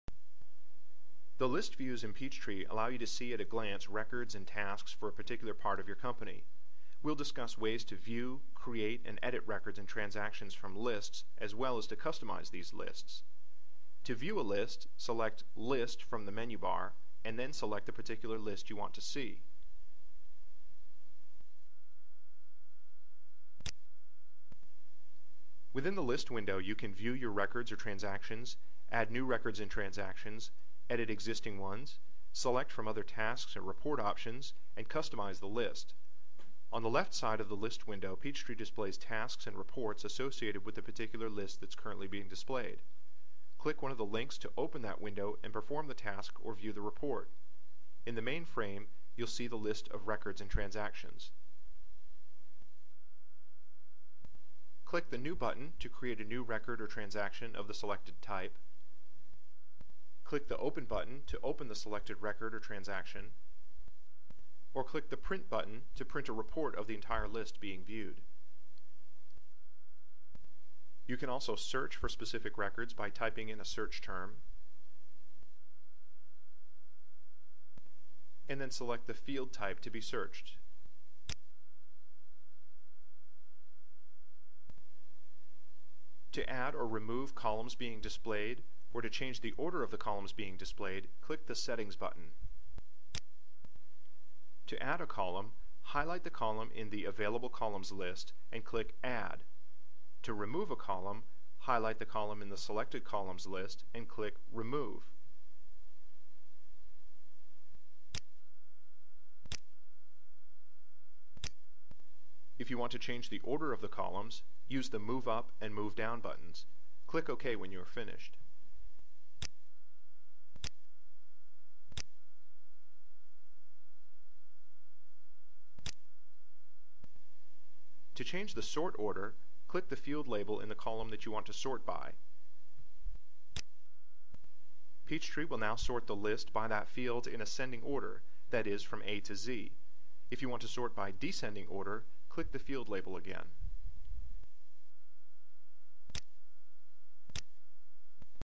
List Views Tutorial- Sage Peachtree: Basics for New Users
for more information on Sage Peachtree.

For more information or questions about Sage Peachtree Support and Training please

Learn how to utilize list views in Sage Peachtree to make record keeping easier.